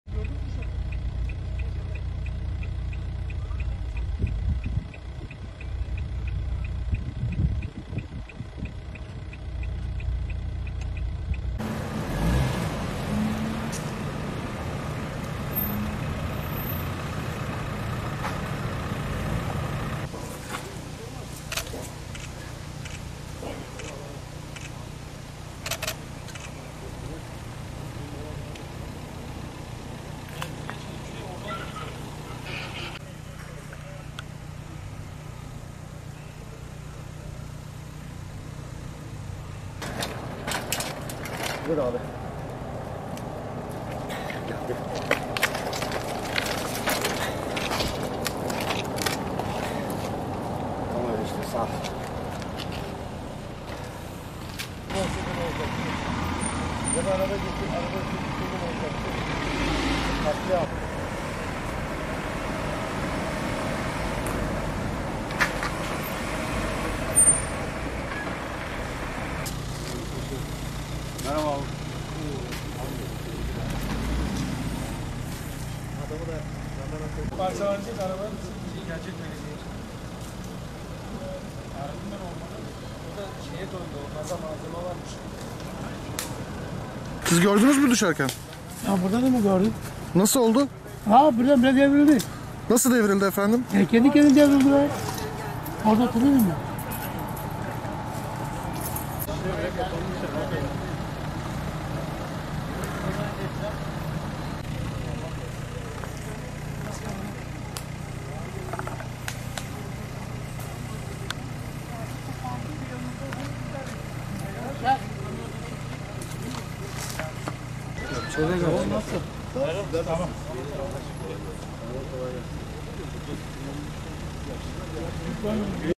Lozan Caddesi'ne ağaç devrildi!
Edirne'nin Karaağaç Mahallesi'yle bağlantısını sağlayan tarihi Lozan Caddesi'nde Galadiçya cinsi ağaç yola devrilirken, şans eseri olay anında kimsenin olmadığı o an bir aracın güvenlik kamerasına anbean yansıdı...